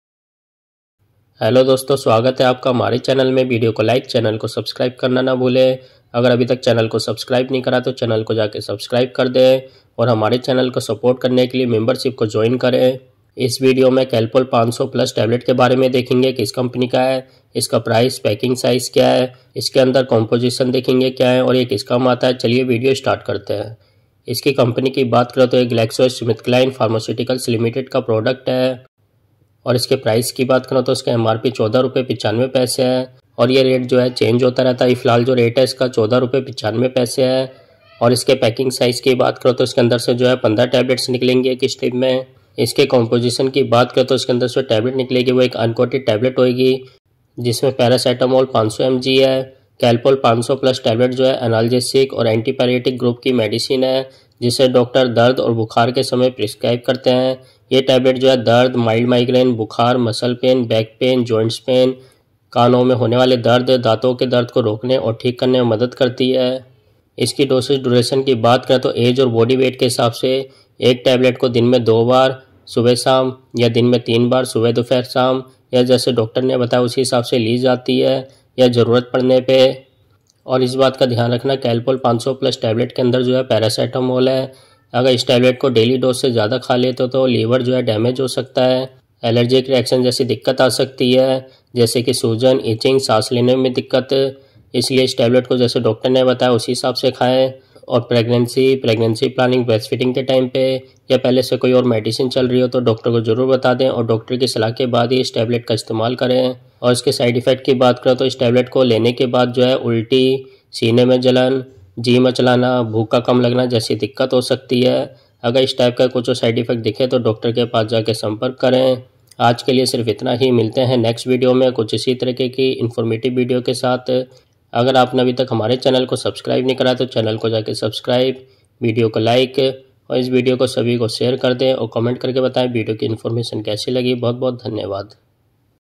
Calpol | Paracetamol Fast Relief Tablets 500 mg Uses In Hindi | Calpol 500 + Tablet Uses In Hindi
Calpol 500 + Tablet (15'S) :- View Company Name, Price , Uses , Dosage, Duration, Side Effects, Composition

        👇All Substitutes For Calpol 500 + Tablet👇
      - Dolo 500 Tablet
      - Crocin 500 Tablet
      - Paracip 500 Tablet
      - P 500 Tablet
      - Pyrigesic 500 Tablet
      - Doliprane 500 Tablet
      - Pacimol 500 Tablet

- Category  :-
   Analgesic & Antipyretic Medicine (Fever , Pain ,Migraine, Headache , Toothe Ache , Muscle Pain , Body Pain , Joints Pain )